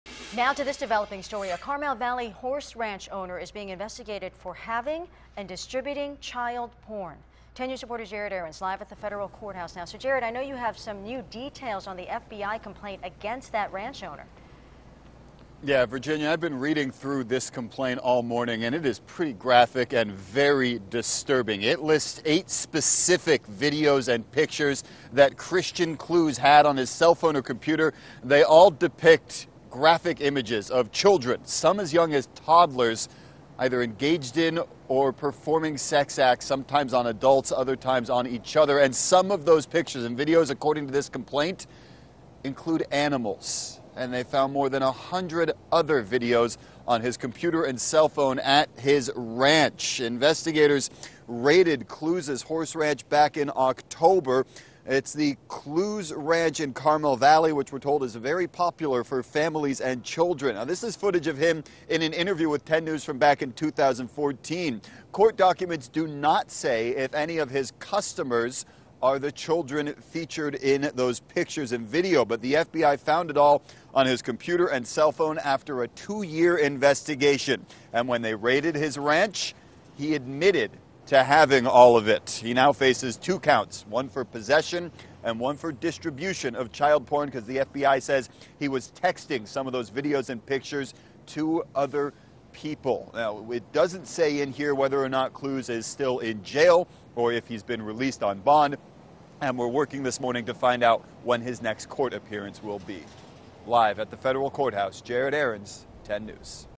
Carmel Valley horse ranch owner accused of child porn
Christian Clews owns Clews Ranch.